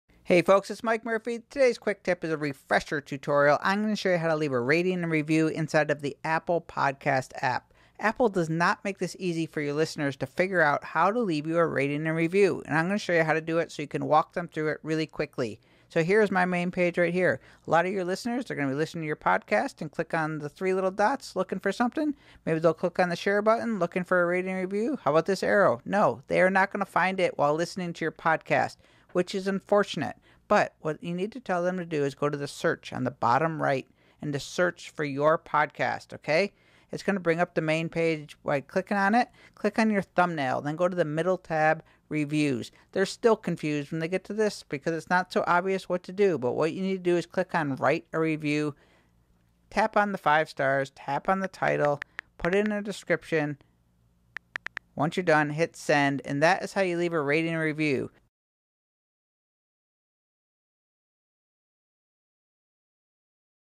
How To Leave a Rating & Review in Apple Podcast App
Quick Tip: How to leave a rating and review for your favorite podcasts in the Apple iOS Podcast App

Warning: @Apple does not make this easy or intuitive for listeners so many people just skip it.

To leave R&R (what the 😎 podcasters say).

In the Podcasts App:

1. Search for your Podcast

2. Click on podcast thumbnail (cover art)

3. Scroll to Write a Review.

4. Tap on stars

5. Leave a title

6. Write a description

Not hard to do, just most people should not have to work so hard to figure that out (and most won't, unfortunately for podcasters).